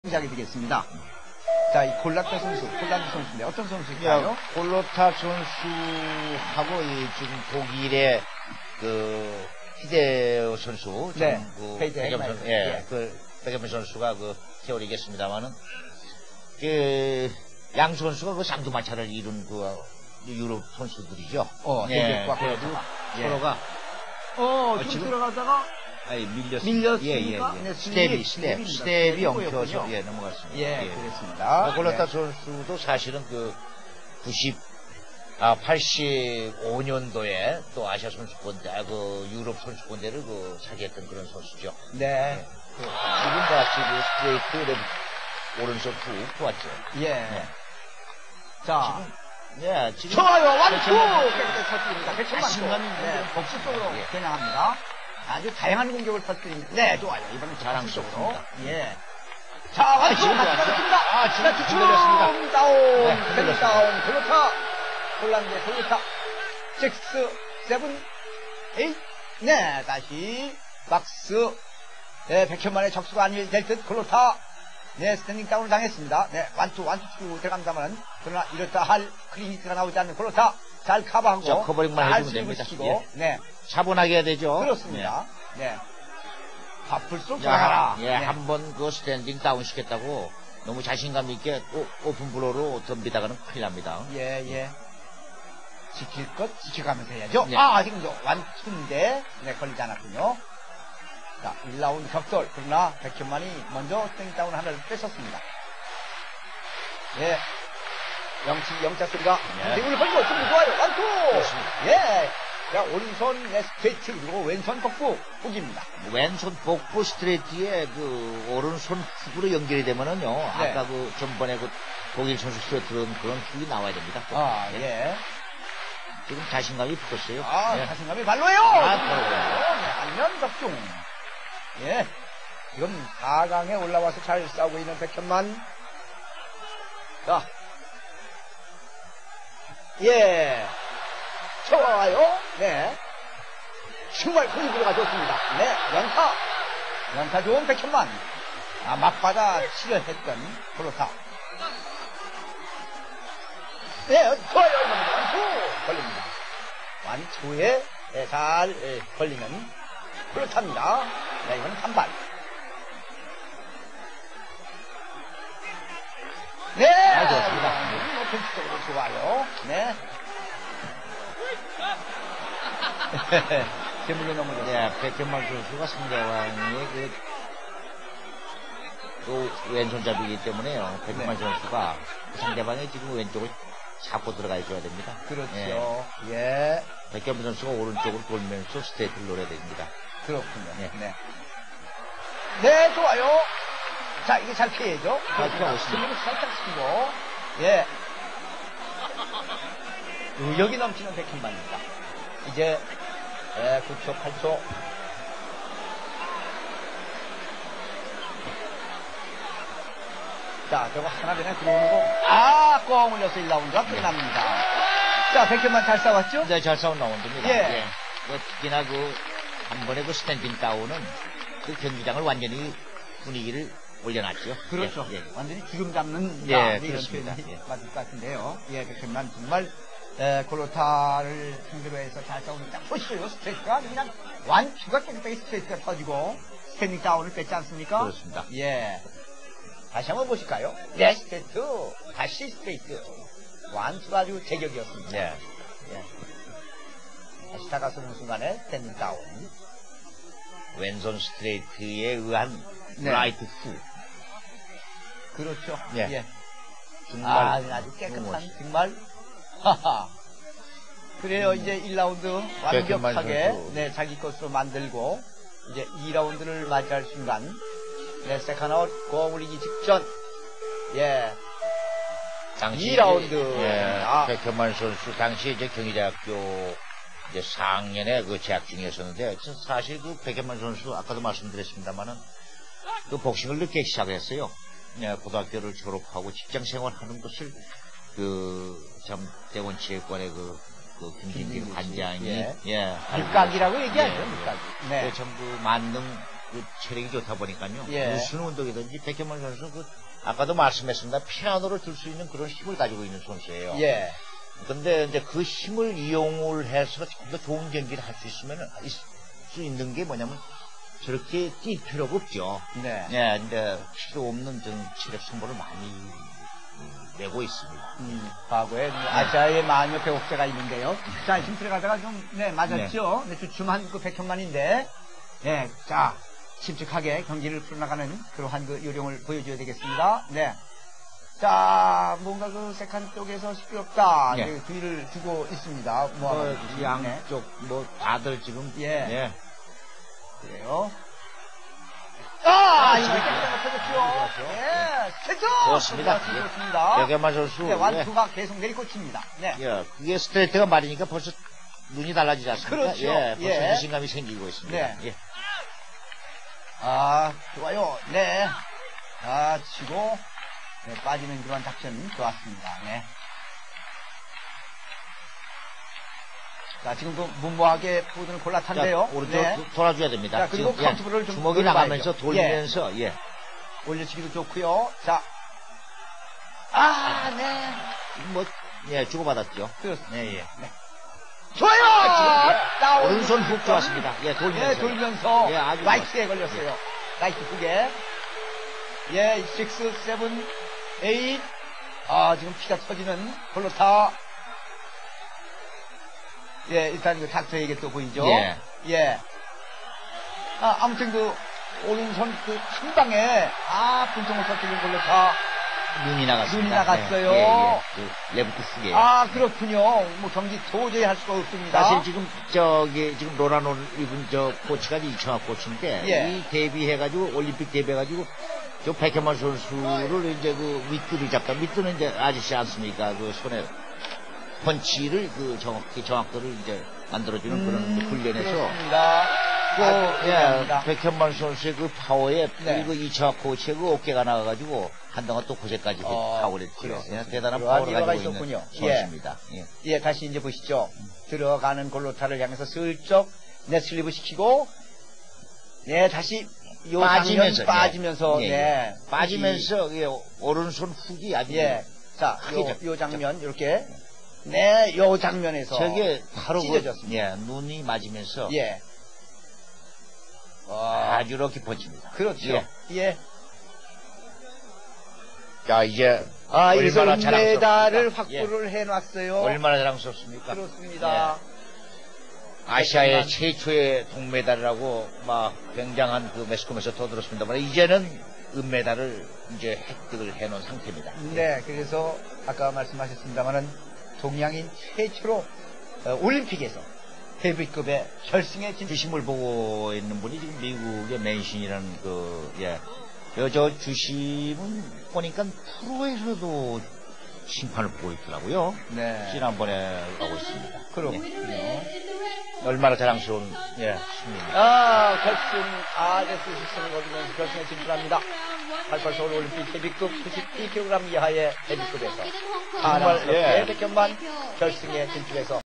Baek Hyun Man vs Andrzej Gołota ( IO Seul 29 IX 1988 - półfinał)
Pechowo zakończona półfinałowa walka Andrzeja Gołoty z reprezentantem gospodarzy.
Igrzyska Olimpijskie w Seulu 1988r.